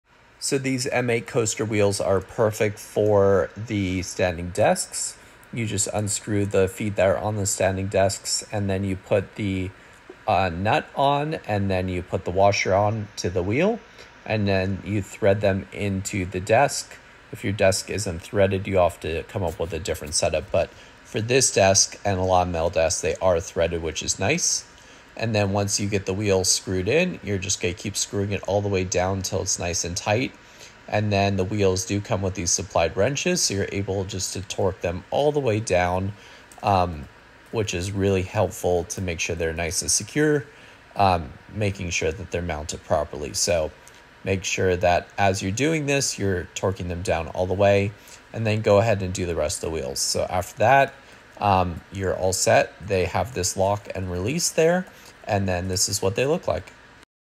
How to Install these M8 Caster Wheels!
We show you how to install these M8 caster wheels on a standing desk.